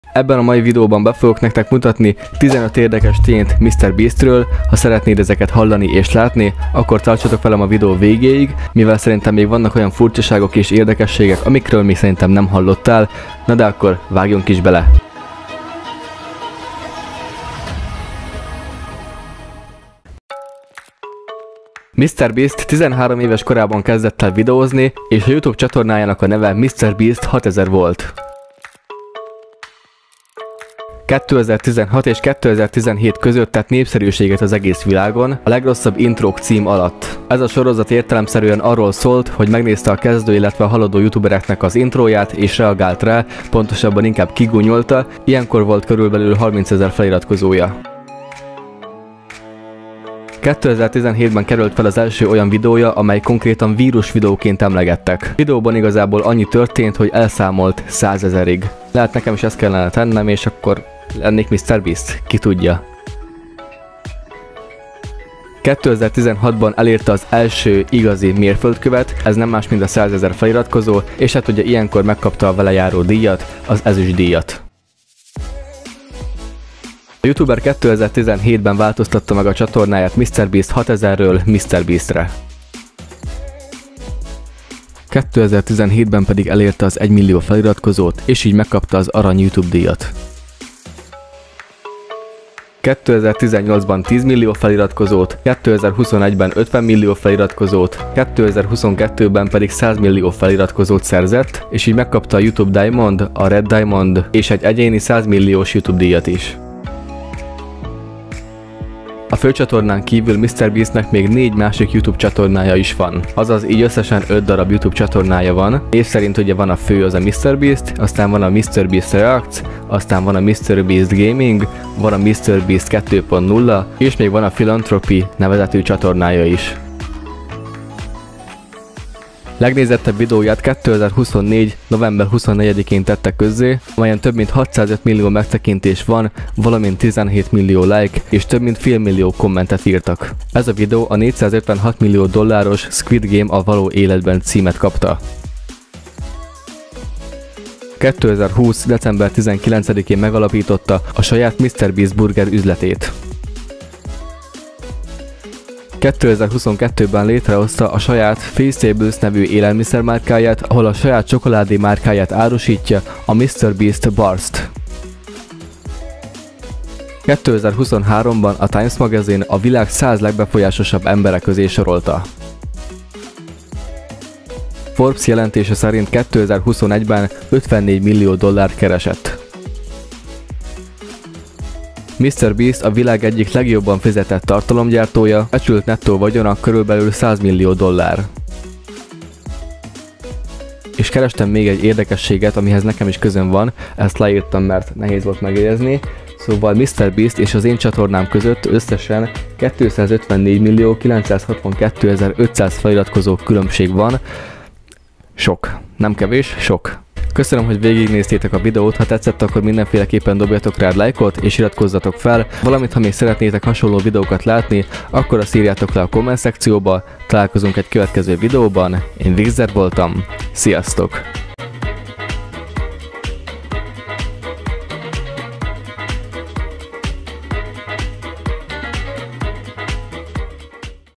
Megdöbbentő Tények Mr Beast-ről! 🤯
Ebben a videóban 15 megdöbbentő tényt mutatok be @MrBeast-ről.